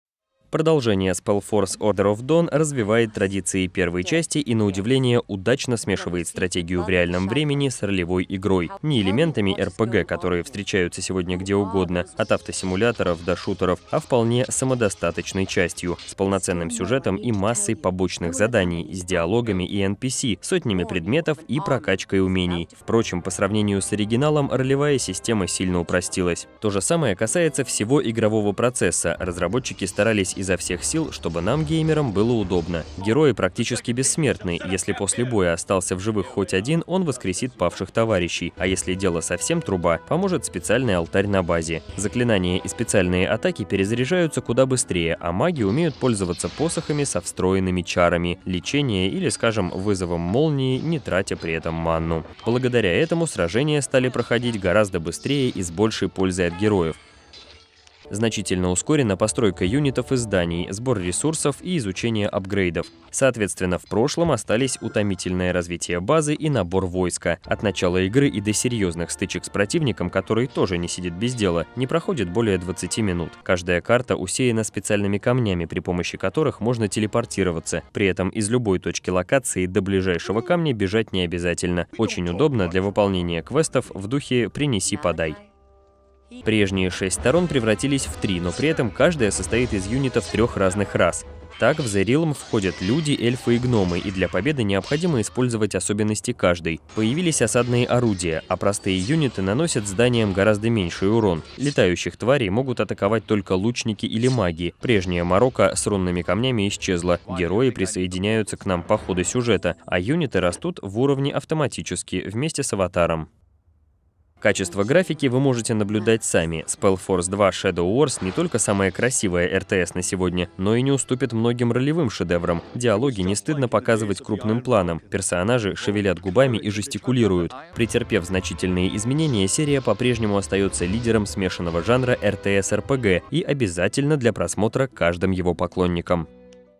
Обзор Spellforce 2 Shadow Wars (2006г)
Обзор Spellforce 2 Shadow Wars в котором ролевая система сильно упростилась по сравнению с оригиналом, а простые юниты наносят зданиям гораздо меньший урон.